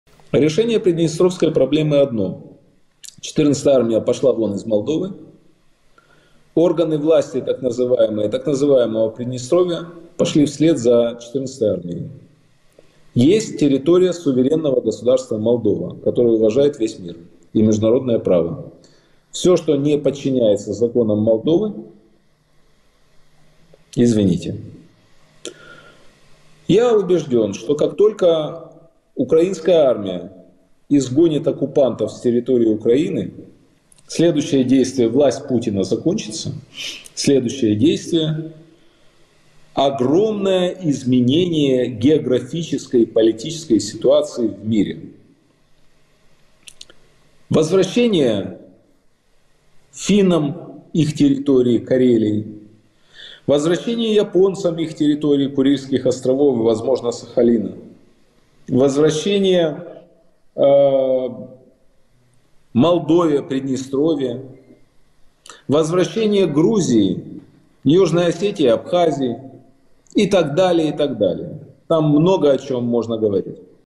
Дмитрий Гордон о решении проблемы Приднестровья
Украинский журналист, Дмитрий Гордон, рассказал, каким он видит решение проблемы Приднестровского региона и находящихся там российских войск. Он поделился своим мнением в эксклюзивном интервью новостному порталу NordNews.